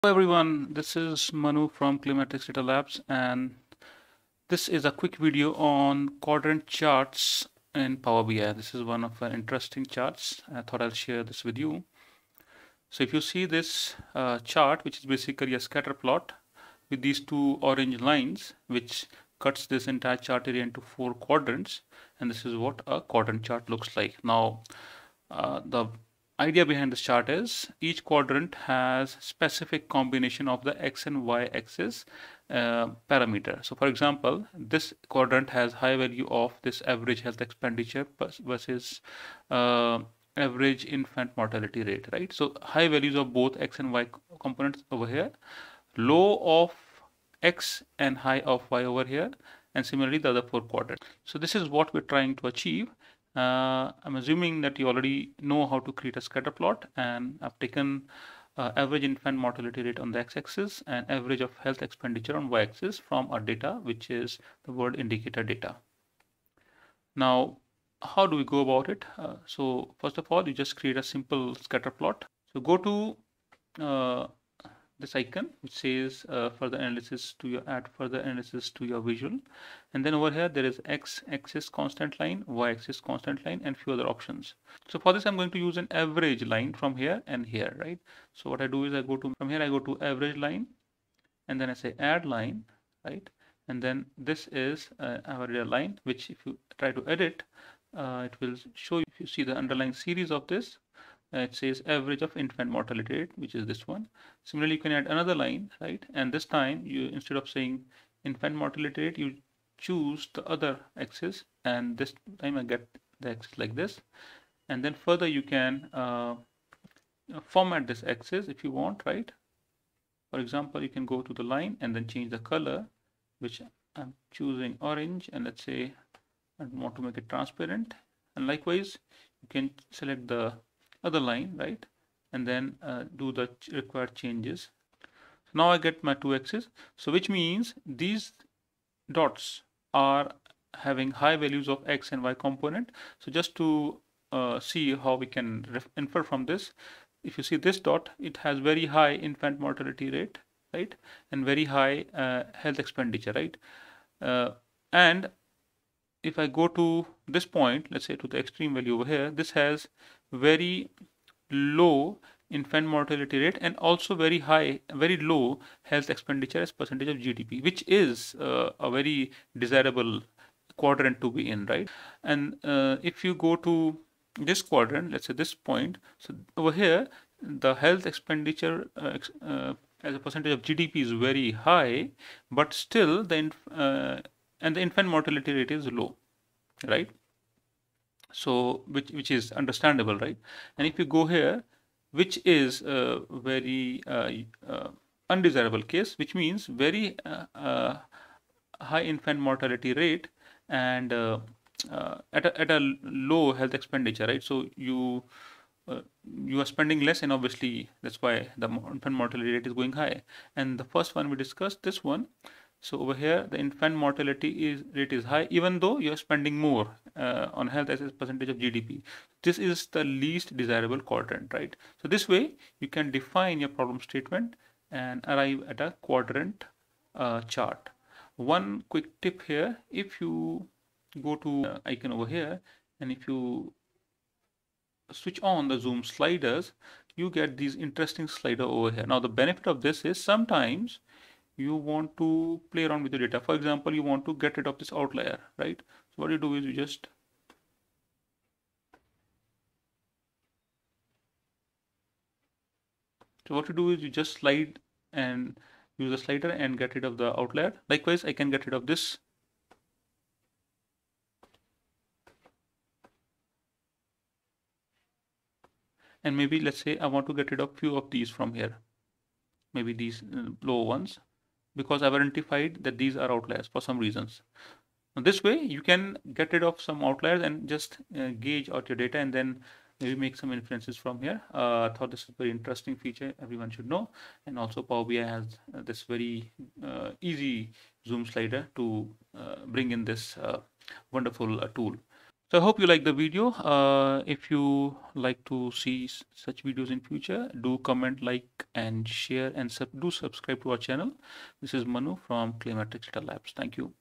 Easily create Quadrant Charts in PowerBI
Quandrant charts are variation of scatter plot with the plot area divided into 4 quadrants buy a horizontal and vertical line. This chart is very useful in understanding the underlying data.

We have some interesting easy to follow certification courses on PowerBI, Tableau, Python, Excel and more.